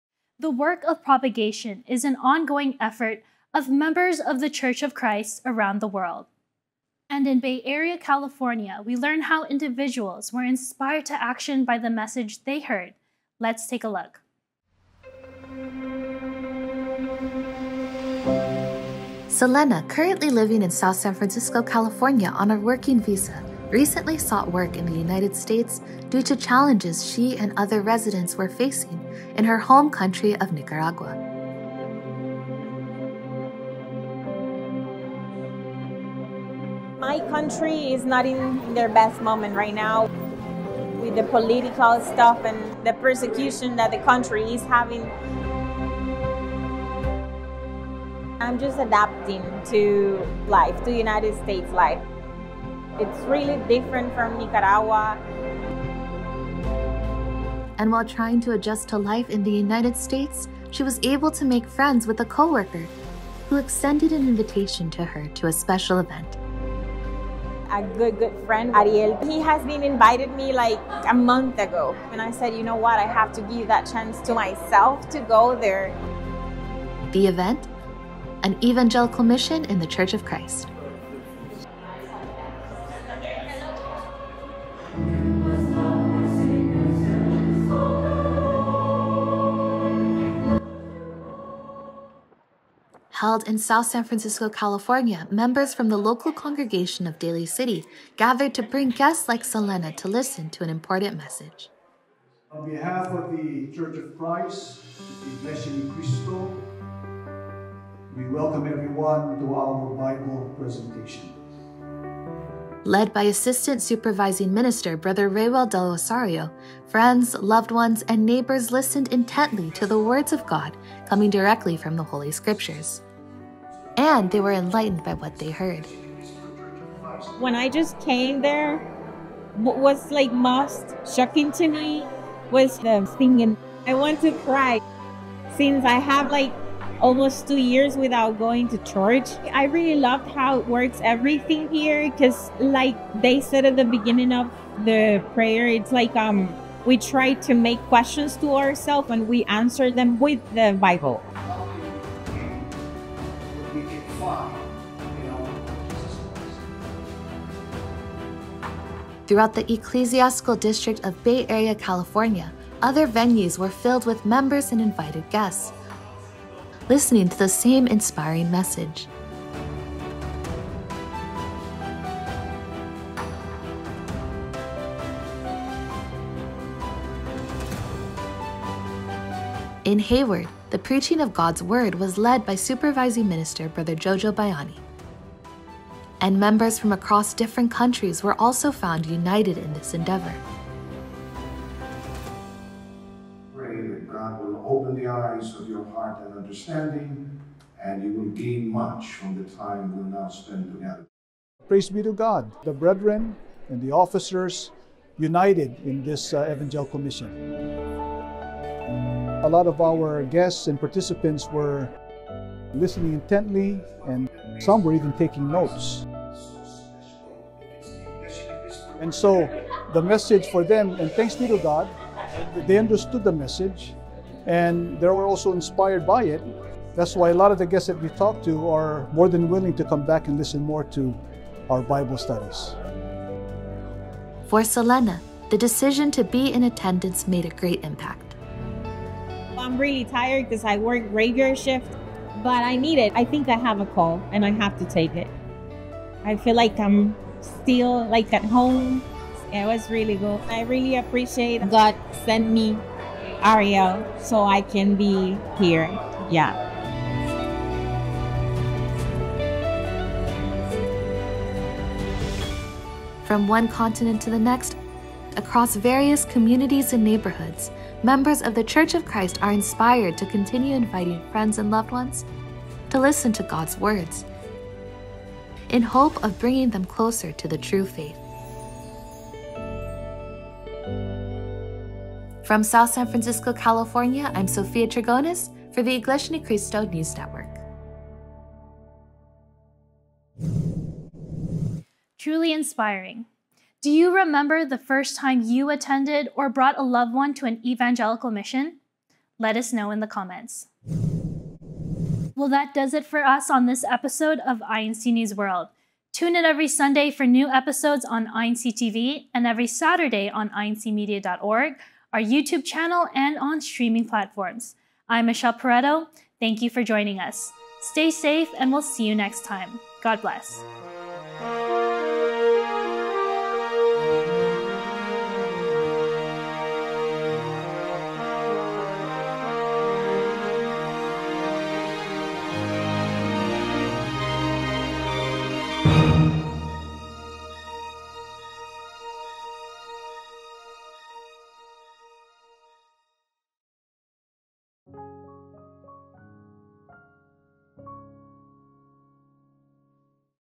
Residents and Guests Inspired by God's Words in Bay Area, California | INC News World
Through the preaching of God's words, guests and neighbors are enlightened in Bay Area, California as part of one of the locations of a worldwide evangelical mission.